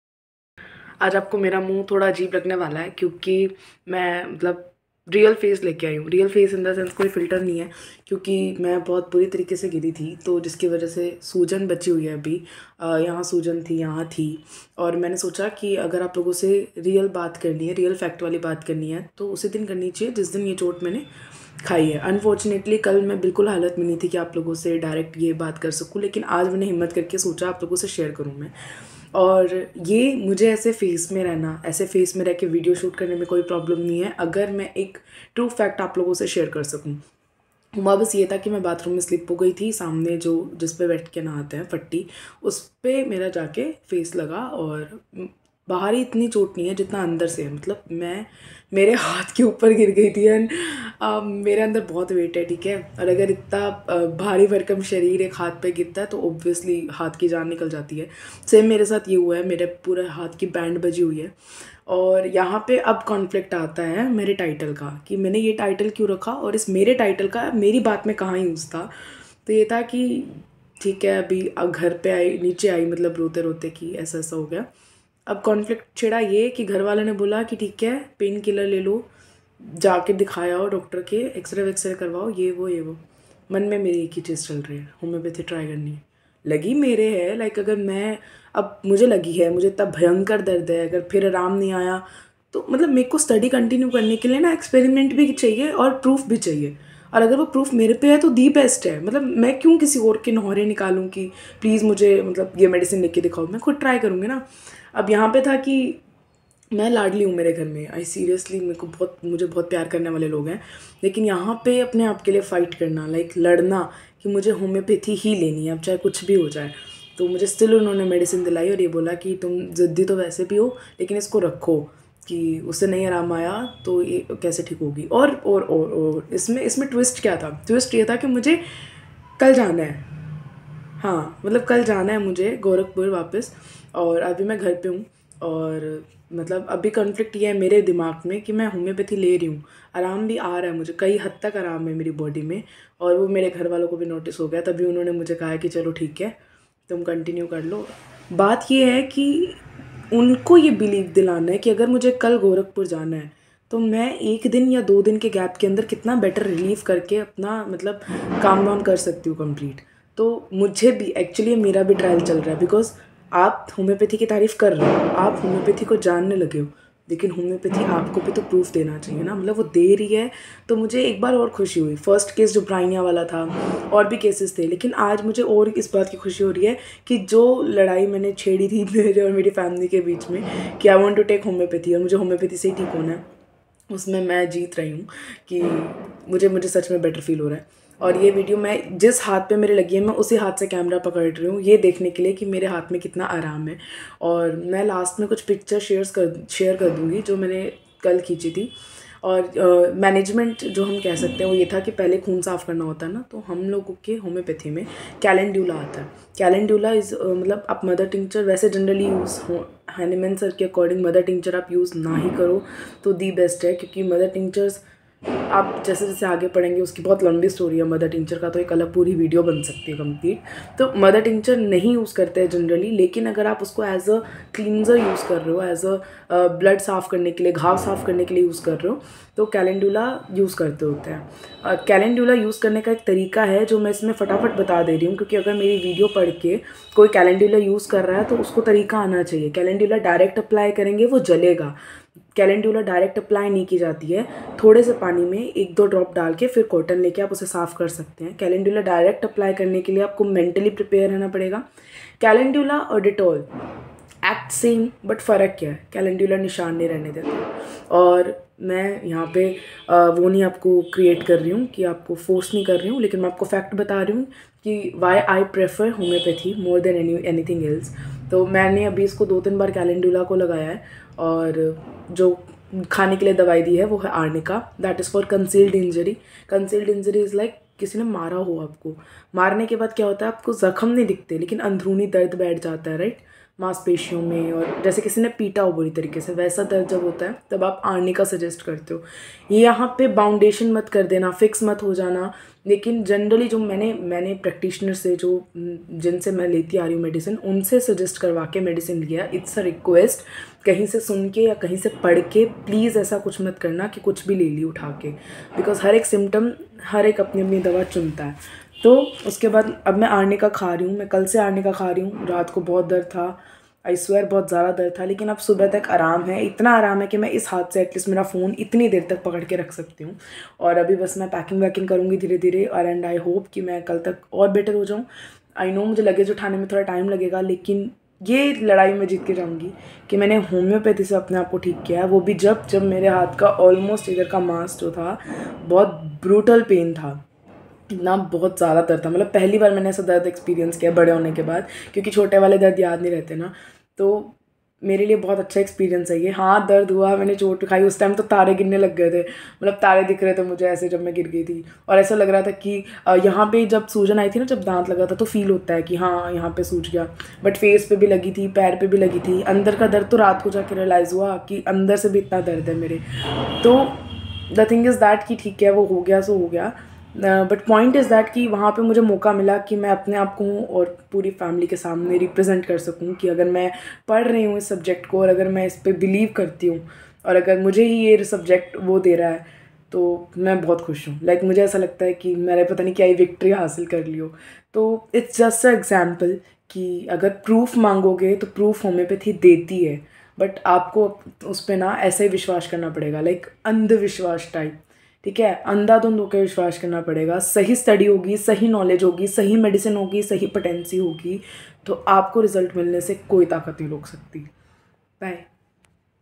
"LADAI ASAAN NHI HOGI" shuruwat kahi na kahi se to krni hogi, ladai asaan nhi hogi🤕💪
ladai asaan nhi hogi...
phle khud se,
fr ghar walo se,
fr maholle se
or fr Puri duniya se ladni hogi.
ye Homoeopathy ko aage leke aane ki ladai...